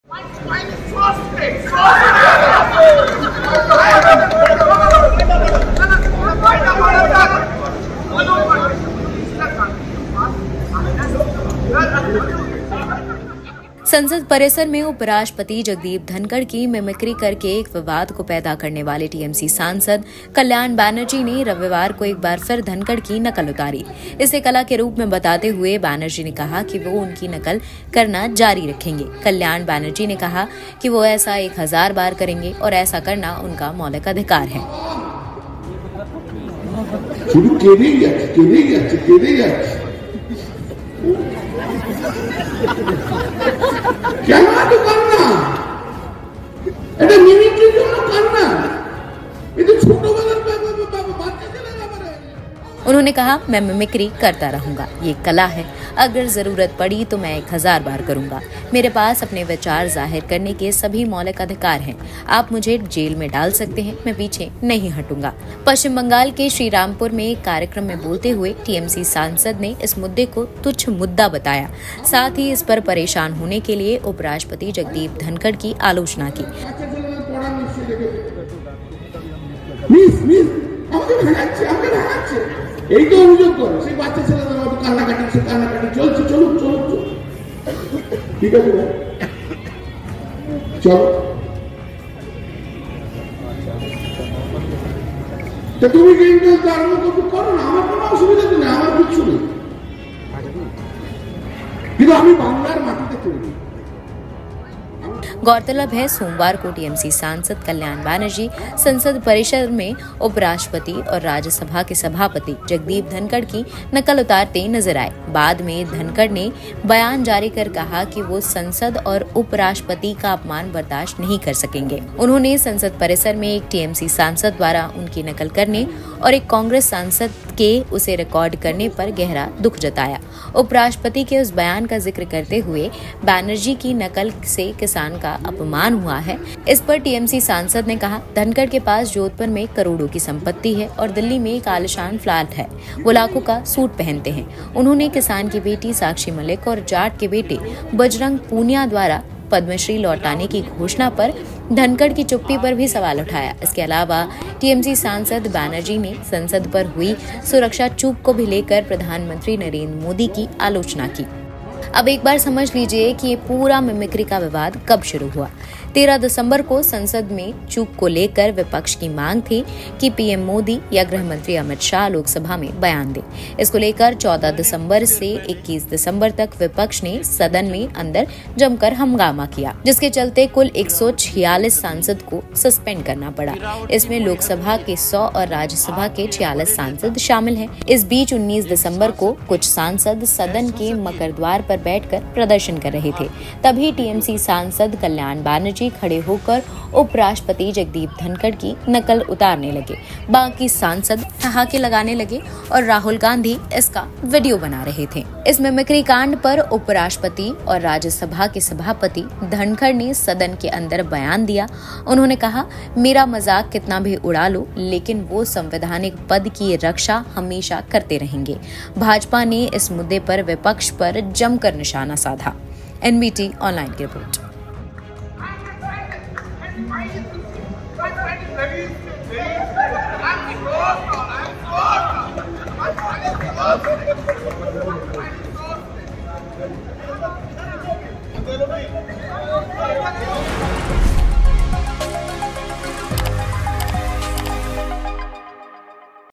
Dhankhar Mimicry: TMC सांसद Kalyan Banerjee ने फिर की मिमिक्री,‘हजार बार ऐसा करूंगा'
Dhankhar Mimicry: TMC सांसद Kalyan Banerjee ने फिर की मिमिक्री,‘पीछे नहीं हटूंगा, हजार बार ऐसा करूंगा'

संसद परिसर में उपराष्ट्रपति जगदीप धनखड़ (Jagdeep Dhankhar) की मिमिक्री (Mimicry) करके एक विवाद को पैदा करने वाले टीएमसी (Trinamool Congress) सांसद कल्याण बनर्जी (Kalyan Banerjee) ने रविवार को एक बार फिर धनखड़ की नकल उतारी. इसे कला का रूप बताते हुए बनर्जी ने कहा कि वह उनकी नकल करना जारी रखेंगे. कल्याण बनर्जी ने कहा कि वह ऐसा एक हजार बार करेंगे और ऐसा करना उनका मौलिक अधिकार है